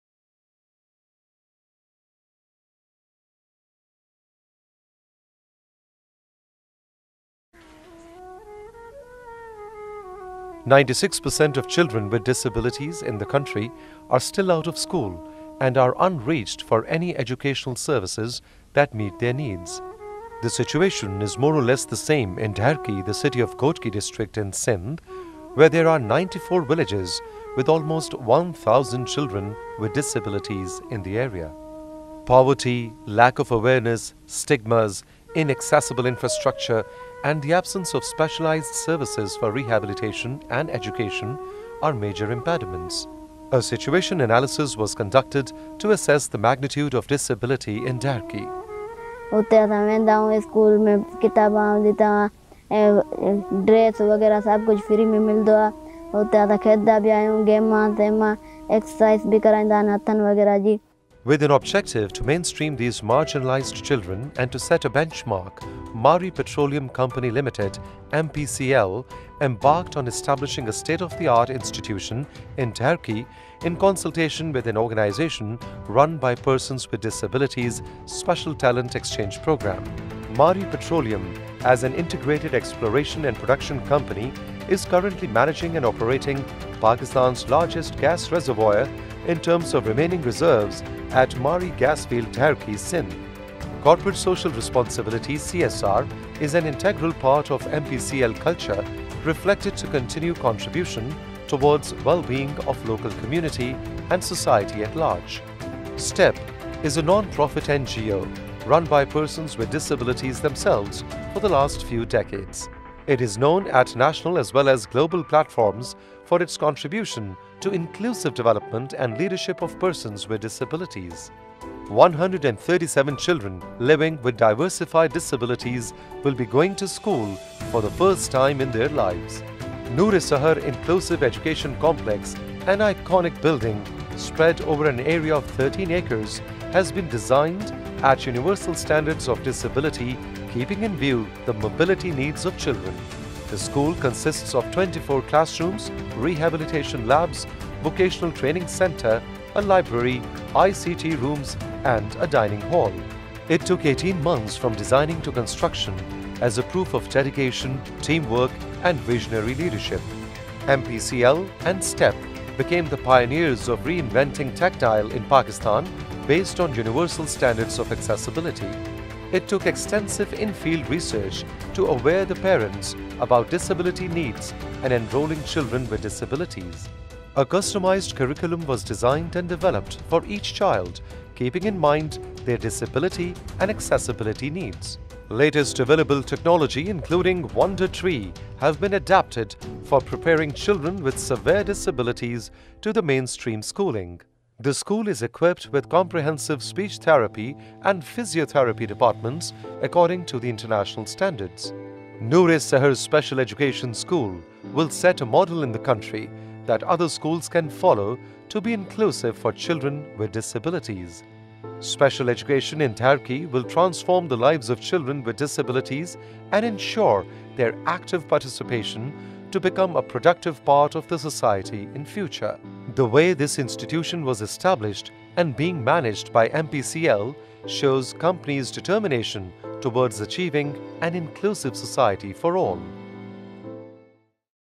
Noor e Seher Special Education School Daharki Sindh with Sign Language
Noor-E-Sehar Special Education School, Daharki
This inclusive school has been designed on Global Accessibility Standards, providing contemporary rehabilitation labs, cutting-edge assistive devices, and an internationally recognized curriculum that would prove to be a catalyst in the cognitive development and social transformation of children with diverse disabilities.
The school has been made operational since January 2020. A total of 131 students have been enrolled in the school with diverse disabilities.
MOU signed between MPCL and STEP (Special Talent Exchange Program) for construction and operations for a period of 3 years.
Construction started in November 2018 and was completed in December 2019.
Provision of state-of-the-art facilities, teaching, and rehabilitation.
The school was inaugurated on 5th October 2020 by MD/CEO MPCL Mr. Faheem Haider.